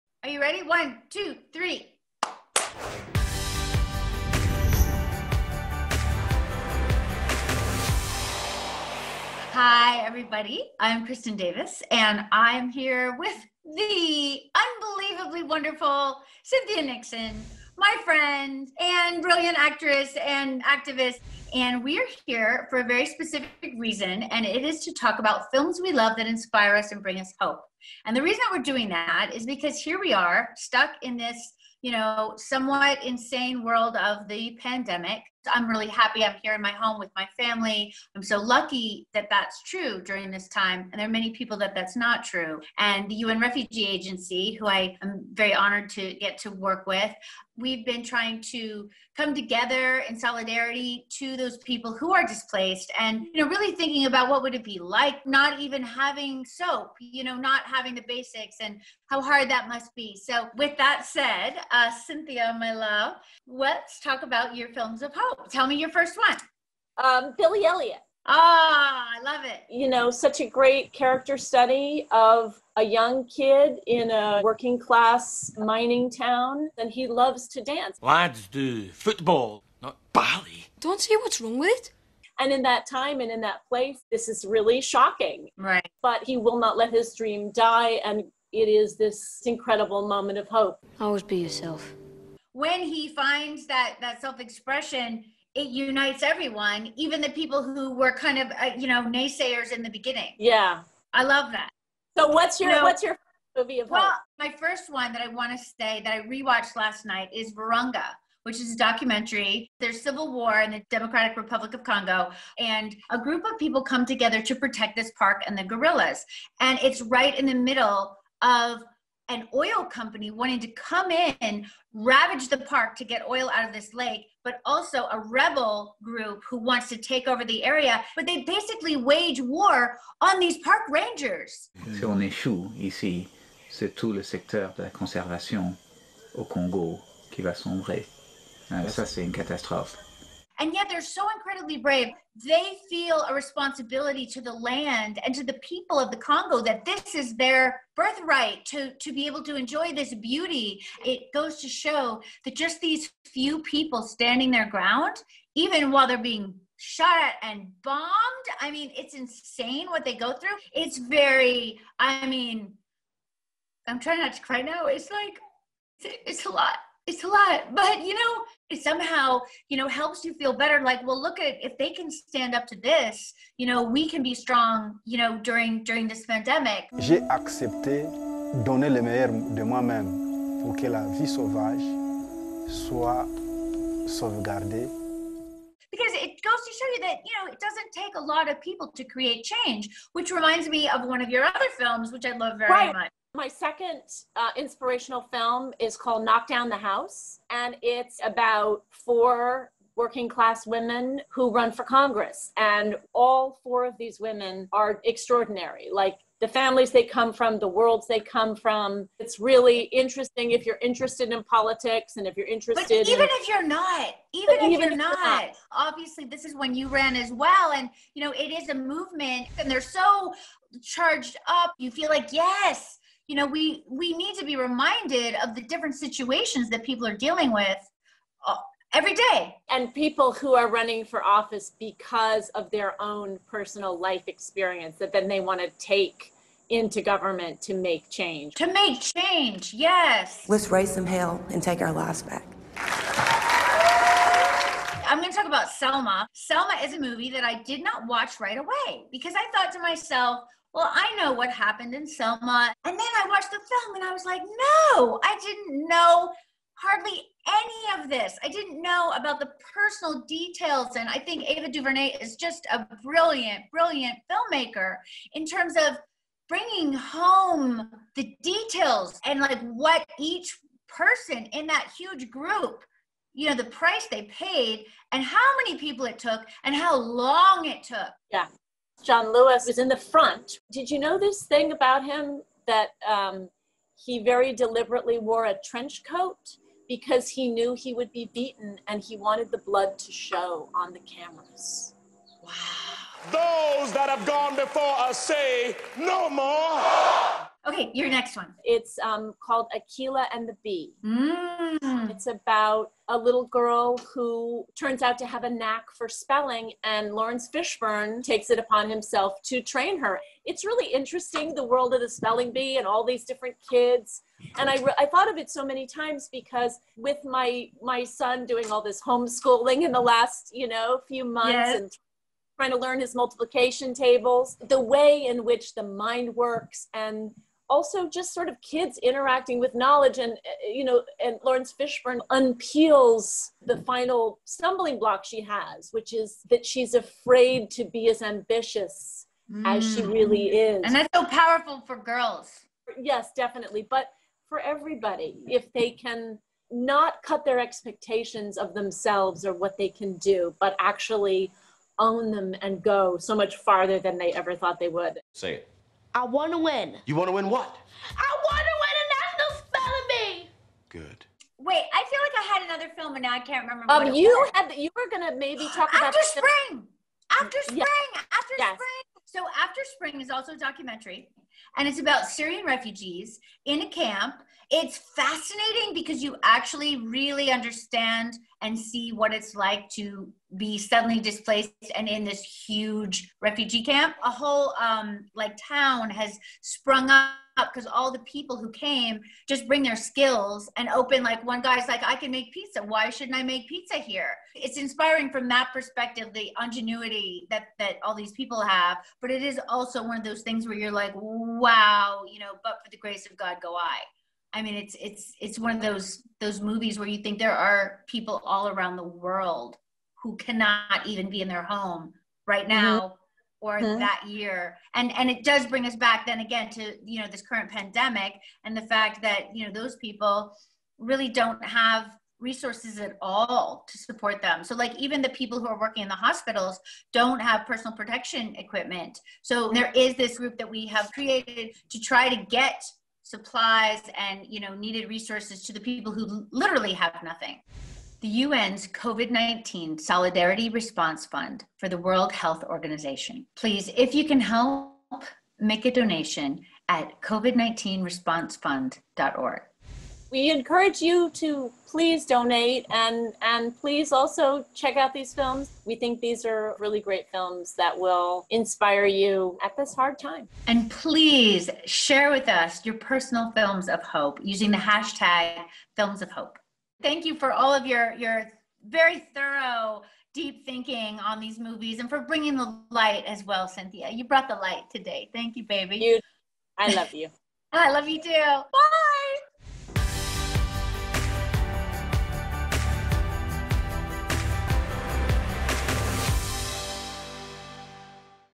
Kristin Davis and Cynthia Nixon Share Their Films of Hope
Longtime friends and former SexandtheCity co-stars KristinDavis and CynthiaNixon join forces with IMDb and the United Nations Refugee Agency (UNHCR) to talk about their favorite uplifting dramas and gripping documentaries that inspire hope in challenging times. The in-depth discussion delves into the power of storytelling and the importance of helping the most vulnerable people around the world, including refugees, who are especially affected by the Covid-19 pandemic.

Cynthia Nixon is moved by the self-expression in BillyElliot, the drive of the women running for office in KnockDowntheHouse, and the keen lesson to embrace your ambition in AkeelahandtheBee. Kristin Davis finds hope in the bravery of park rangers protecting their land in Virunga, the persistence of JohnLewis and all those who marched with him in Selma, and the resolve of the Syrian refugees in AfterSpring.

If you are inspired to help support the cause, please make a donation